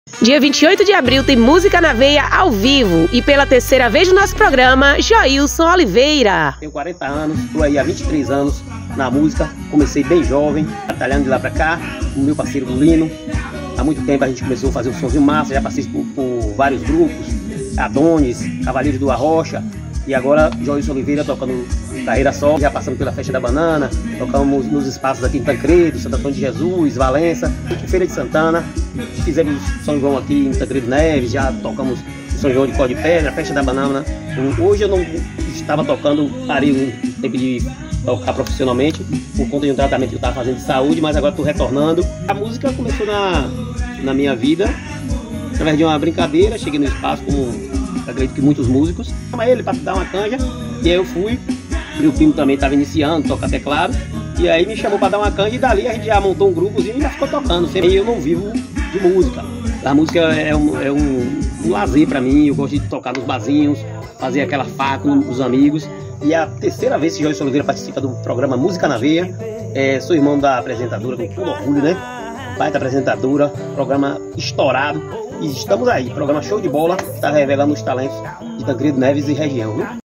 Joilson Oliveira 💥 23 anos de música, sonhos e sucesso! 🎬 Live dia 28.04 às 20hs
O programa Música Na Veia valoriza o seu talento!

Quer participar? Entre em contato pelo Instagram @keilla.oliveeira ou @blogdakeilla

Um beijo da negona 😘